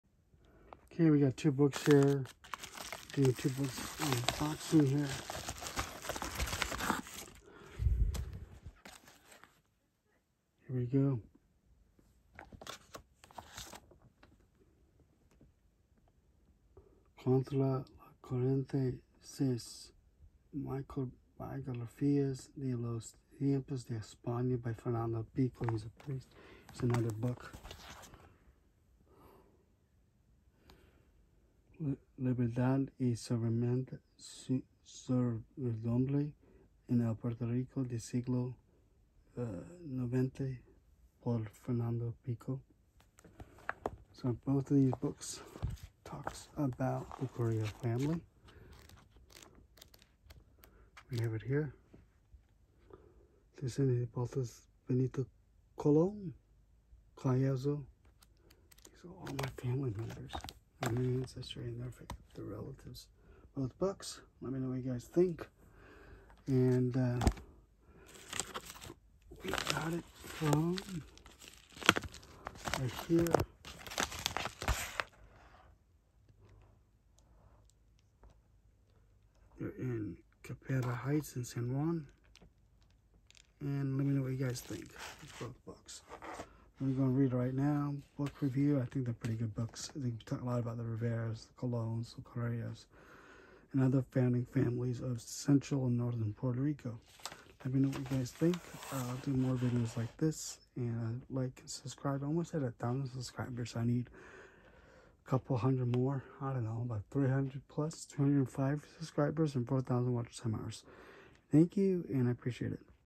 Unboxing And Review Of Books About Puerto Rico
Unboxing two Fernándo Pico’s books abt mini biographies in the Spanish colonial period of Puerto Rico. They are about my ancestors. And many Puerto Ricans are descendants of them also.